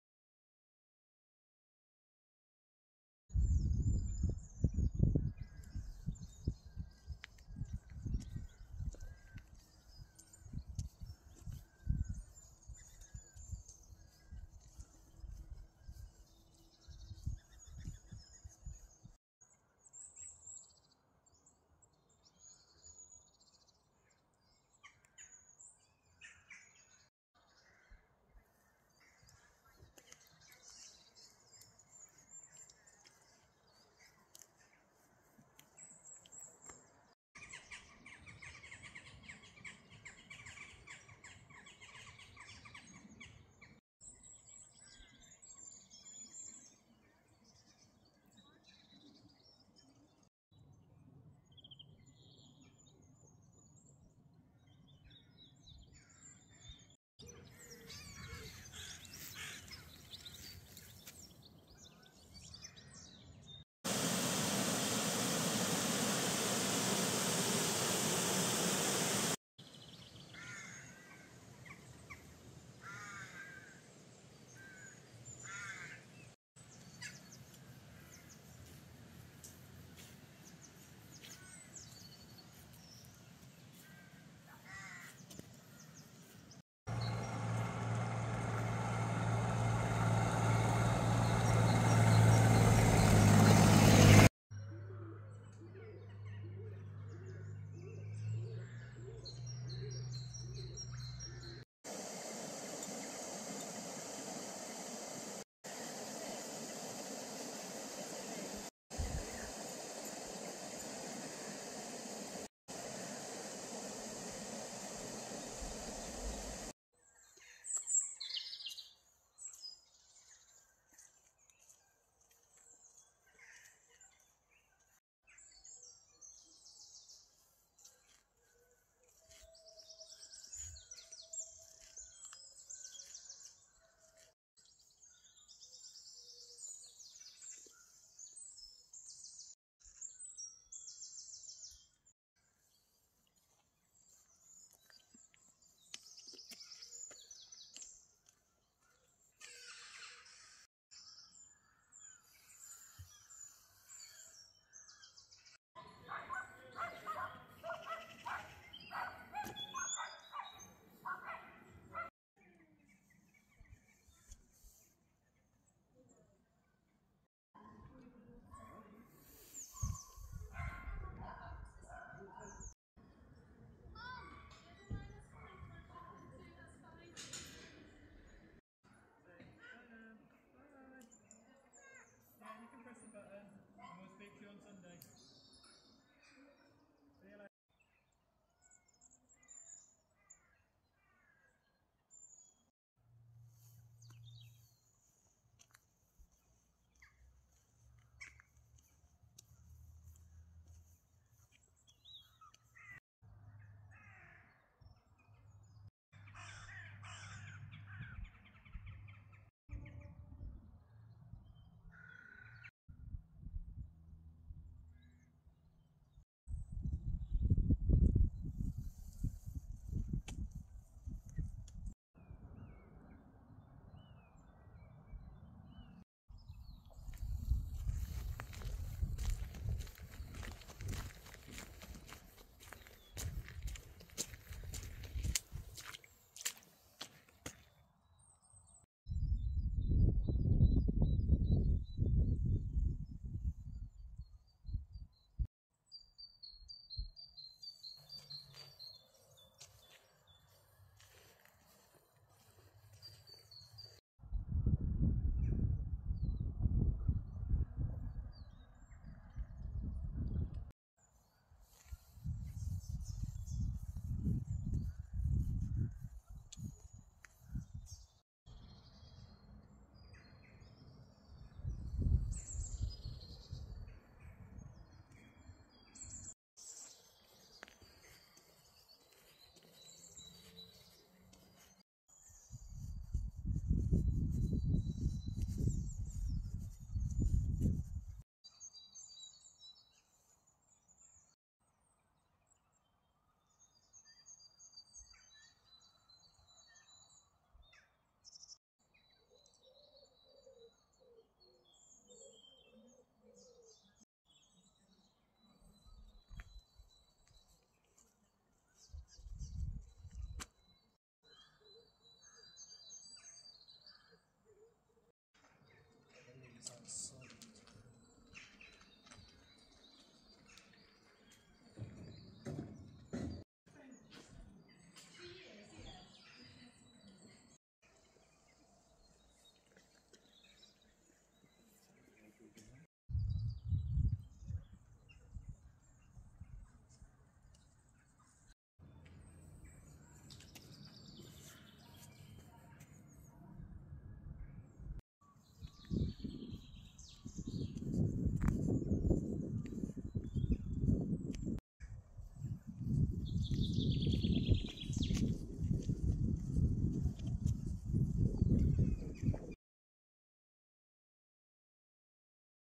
Lydiard Park - A stroll without music
This shows a route around Lydiard Park which is located in Swindon.